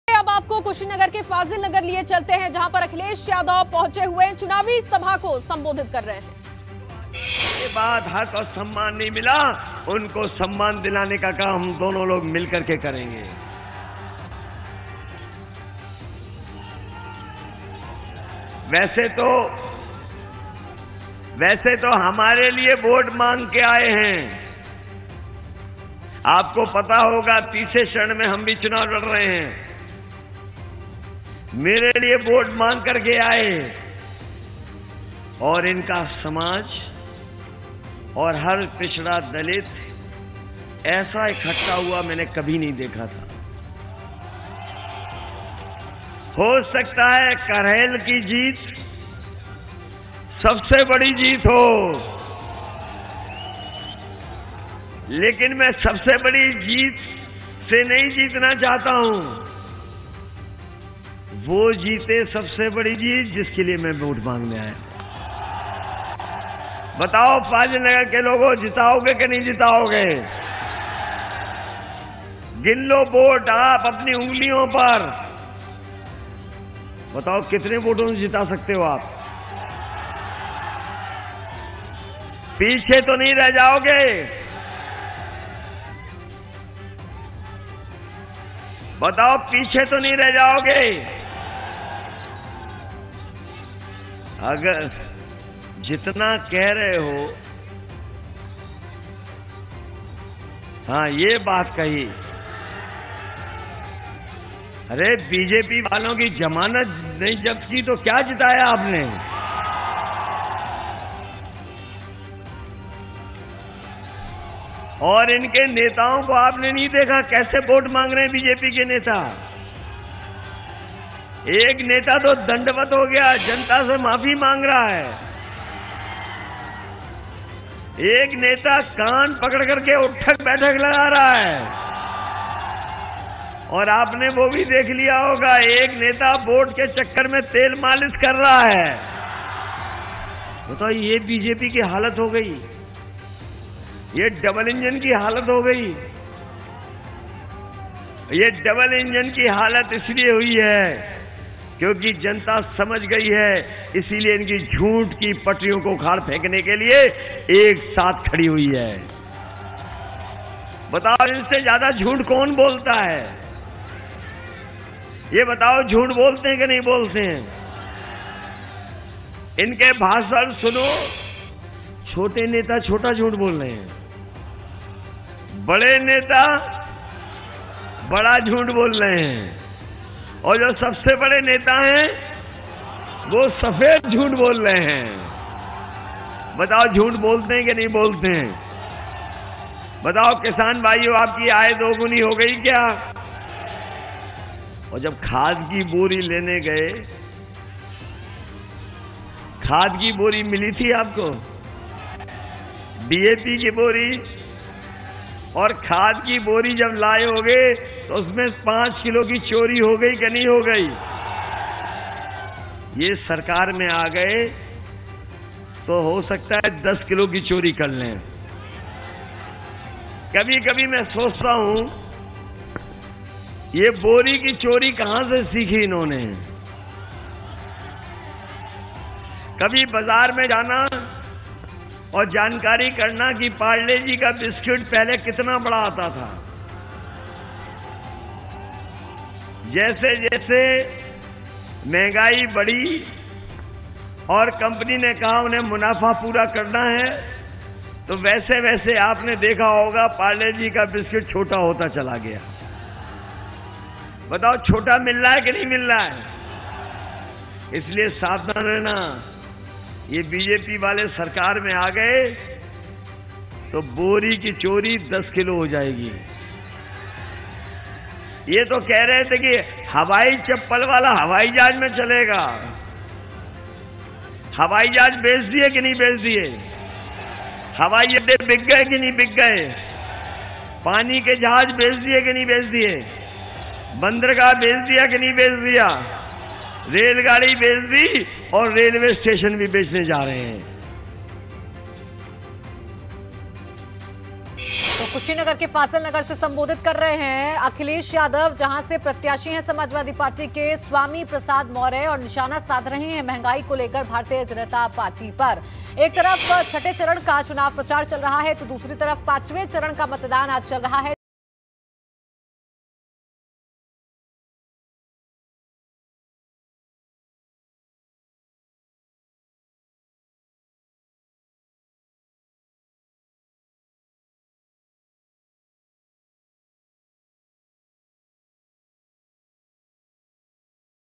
UP 5th Phase Voting: Kushinagar में Akhilesh का हमला, 'BJP ने कहा था नोटबंदी के बाद कालाधन नहीं
UP 5th Phase Voting: Kushinagar  में Akhilesh का हमला, 'BJP  ने कहा था नोटबंदी के बाद कालाधन नहीं बचेगा'। देखिए ये पूरी रिपोर्ट